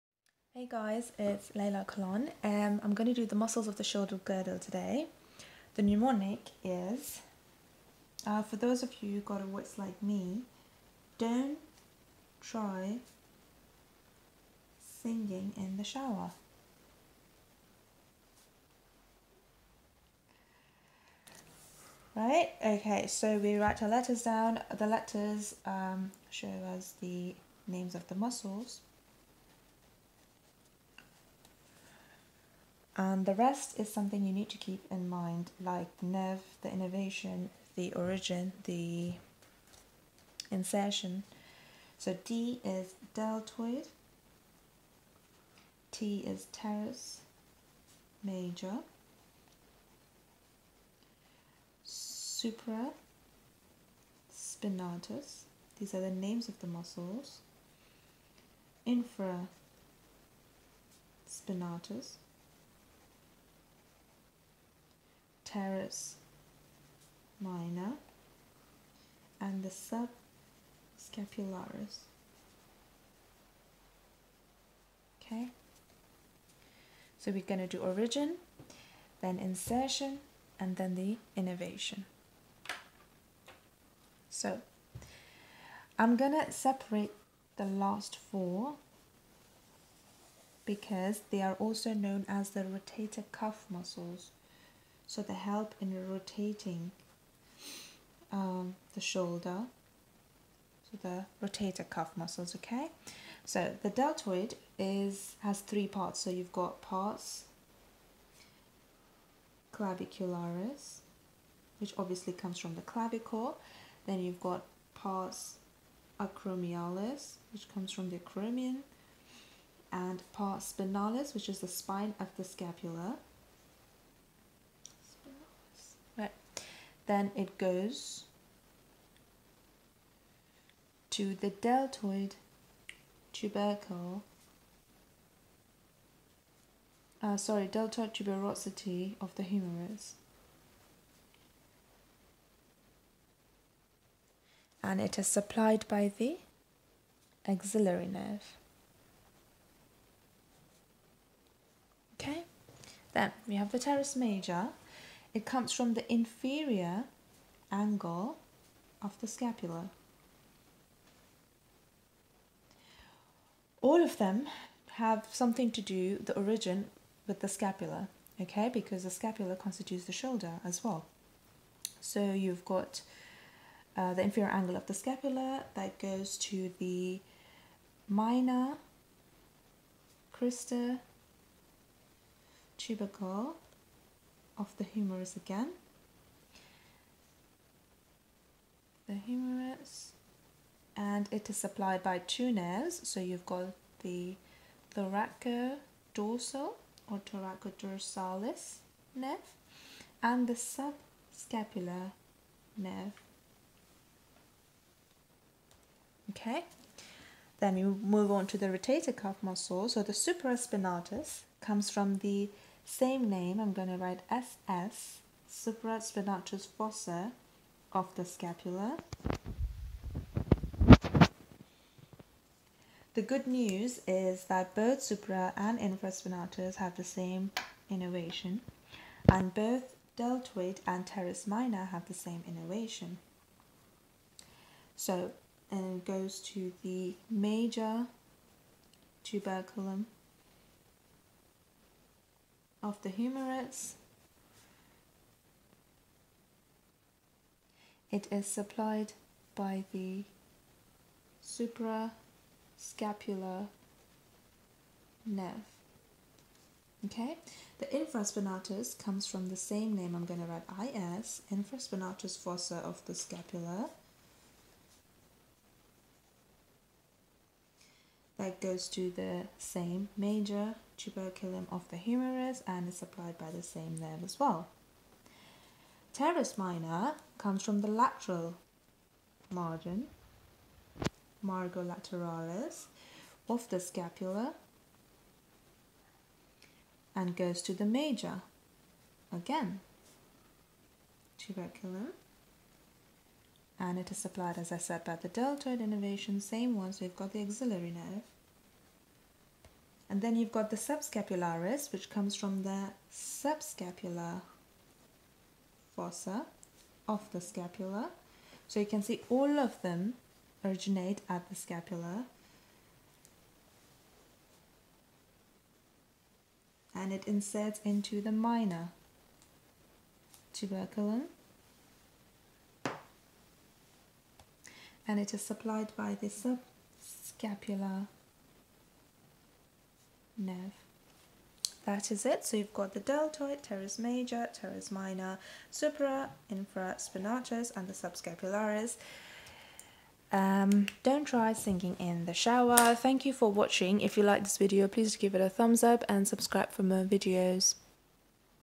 Muscles of The Shoulder Girdle
Learn it the mnemonic way;
Don't Try Singing in The Shower!